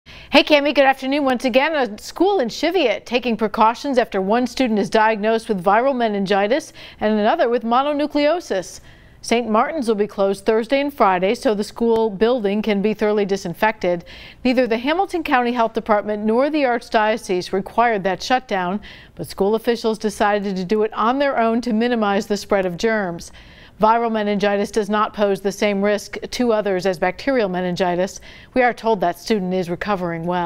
Cheviot school closing for 2 days after students diagnosed with viral meningitis, mono
CHEVIOT, Ohio (WKRC) - A local school is closing for two days to disinfect from contagious germs.

St. Martin in Cheviot is taking precautions after one student was diagnosed with viral meningitis and another with mono. They will be closed Thursday and Friday so the school building can be thoroughly disinfected.

Neither the Hamilton County Health Department nor the archdiocese required the shutdown. School officials decided to do it on their own to minimize the spread of germs.

Viral meningitis does not pose the same risk to others as bacterial meningitis.

Local 12 was told the student is recovering well.